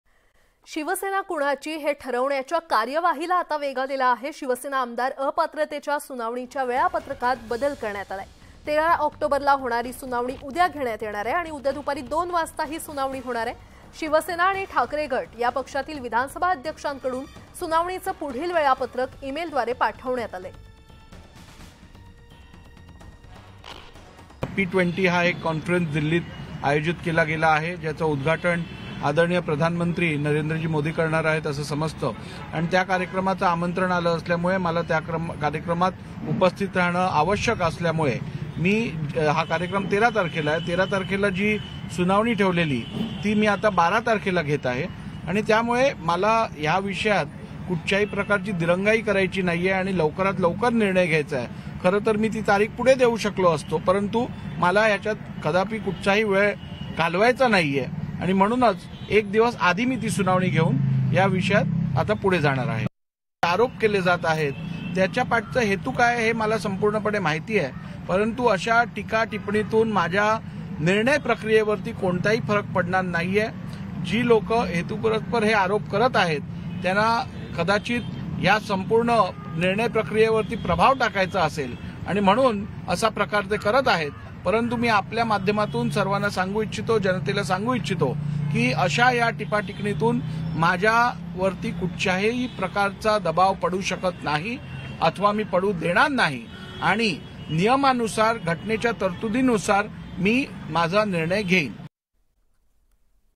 Uddhav Thackeray VS Eknath Shinde | शिवसेना आमदार अपात्र प्रकरणी उद्या सुनावणी | Marathi News
शिवसेना आमदार अपात्रता प्रकरणी विधानसभा अध्यक्ष राहुल नार्वेकर यांच्याकडून वेळापत्रात बदल... १३ तारखेला होणारी सुनावणी आता उद्याच होणार